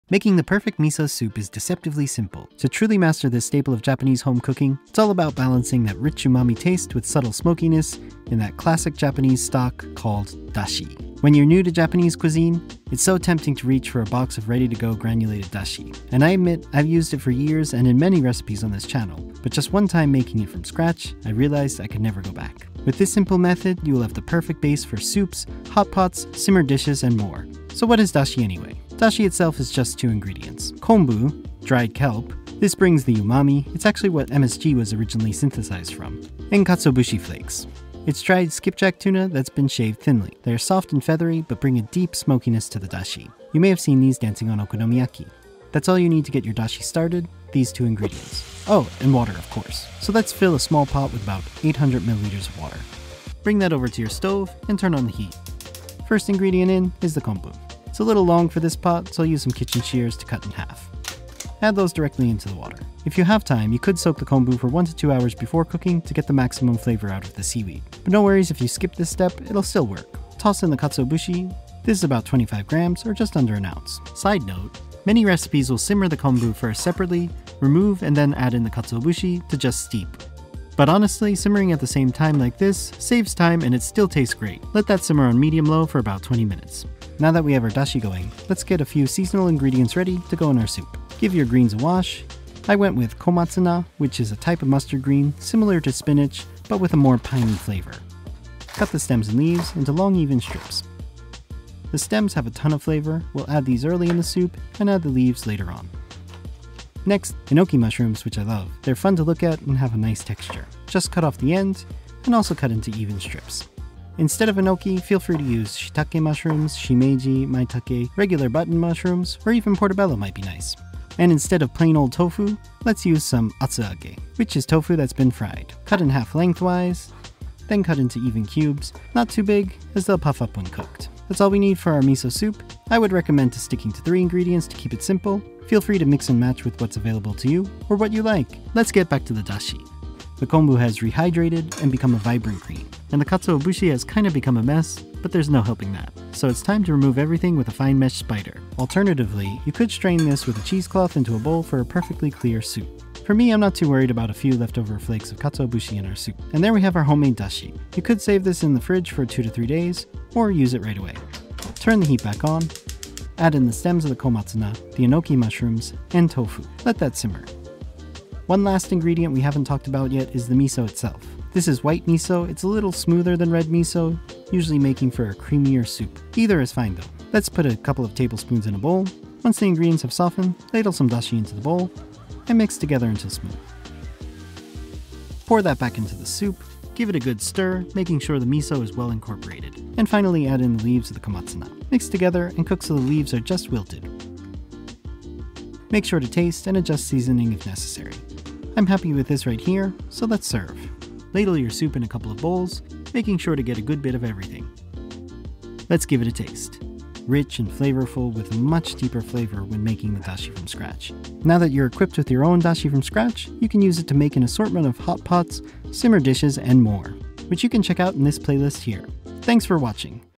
Make BETTER Miso Soup at Home with this ONE Step
Level up your miso soup at home with a dashi stock from scratch. Dashi is the basis for many dishes in Japanese cuisine including hot pots, simmered dishes, and of course miso soup. Once you have a homemade dashi, you’ll never want to go back to the granulated version. It only takes about 20 mins, and you will be on your way to a super umami and flavorful dashi.

Also featured in this video, is miso soup with seasonal ingredients including komatsuna, enoki mushrooms and abura-age (fried tofu). Feel free to sub with other vegetables that you like!

The Ingredients (3-4 servings):

The Dashi:
800 ml/3.5 US cups of water
20g/0.7 oz of kombu (dried kelp)
20-30g/0.7-1 oz of katsuobushi flakes

The soup:
Your choice of mushrooms
Your choice of greens
Your choice of tofu
2 tbsps miso paste

0:00 Intro
0:27 What’s dashi?
0:49 Simmering the stock
1:27 Preparing the soup ingredients
2:19 Skimming the dashi
2:41 Putting it all together
3:21 Time to serve!